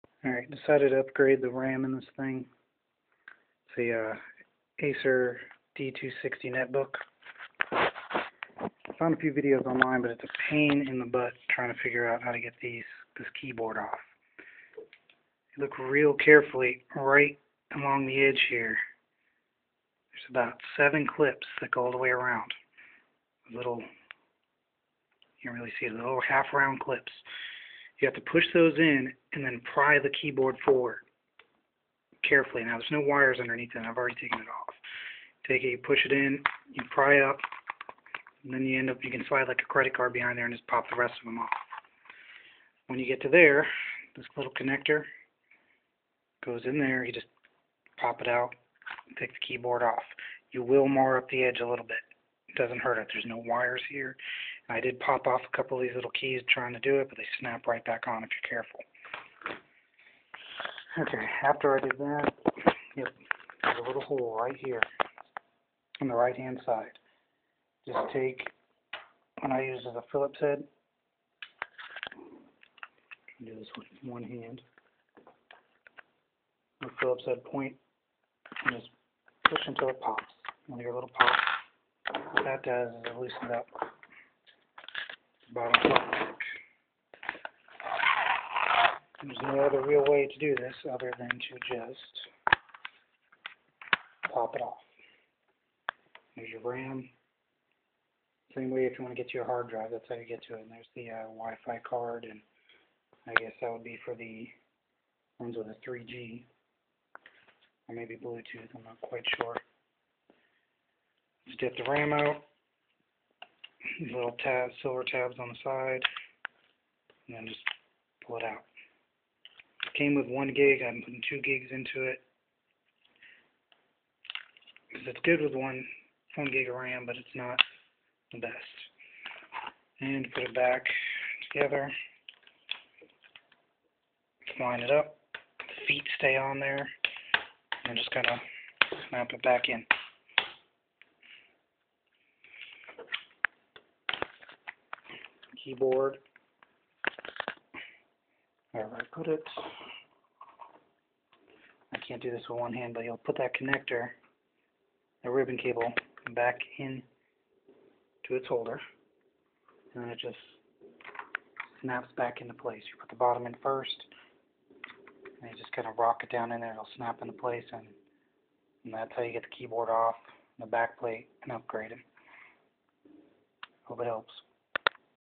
How to access the Ram and Hard Drive on an ACER ASPIRE ONE D260 READ Description 1st!
MAKE SURE YOU HAVE THE SAME MODEL THE ONE I HAVE DOES NOT HAVE SCREWS HOLDING THE BACK ON.

Of all the videos Shown I couldn't find one that shows how to do this with the keyboard already removed.

 I shot this on a Blackberry so forgive the quality.

Getting the Keyboard ribbon back in is a bit difficult as well. Only thing I can say is be patient or find someone with tiny fingers to help.

If you mess up your pc that's on you.